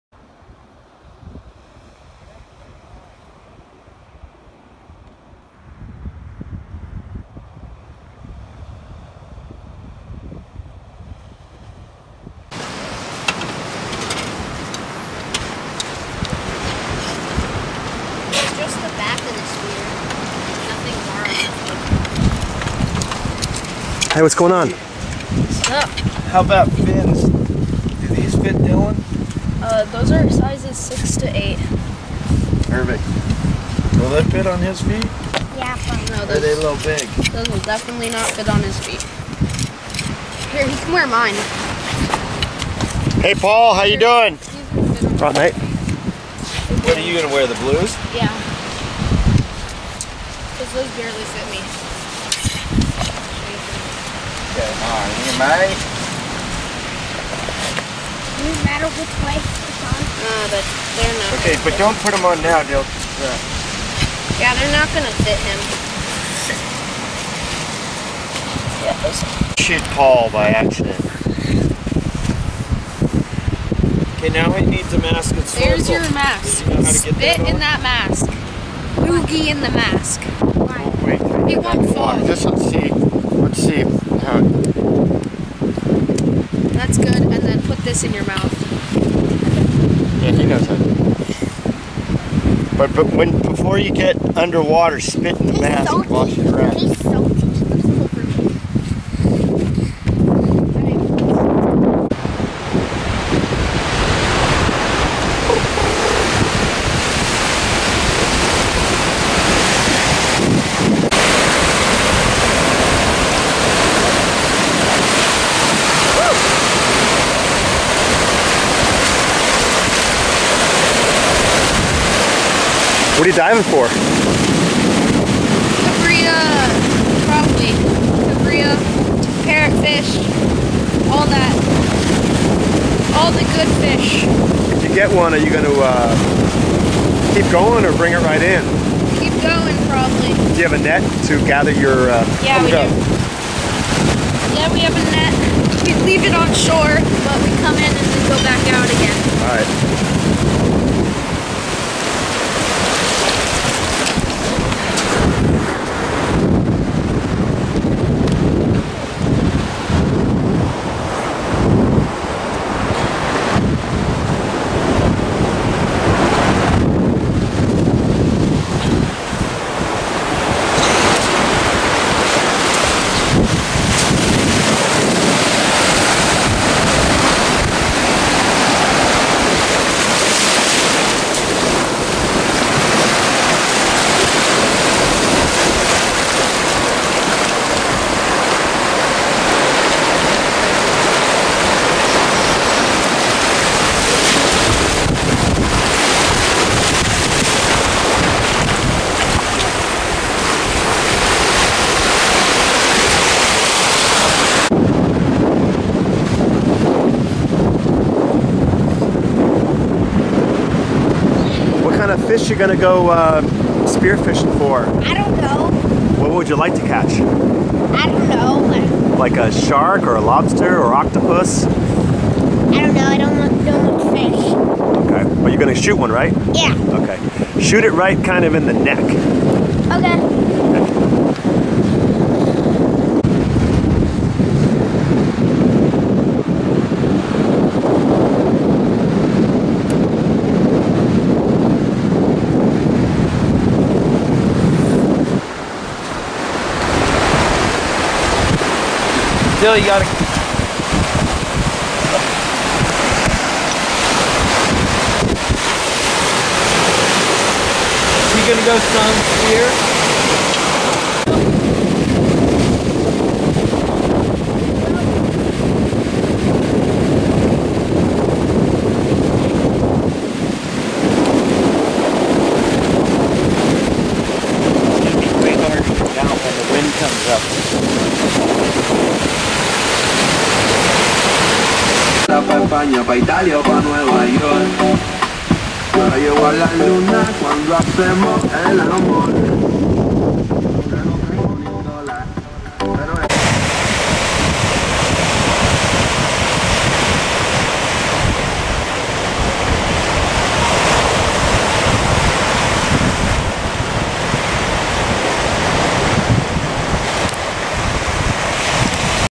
Buceo Cardonal
Dil and Duke go diving for Cabrilla en el Cardonal, Baja California Sur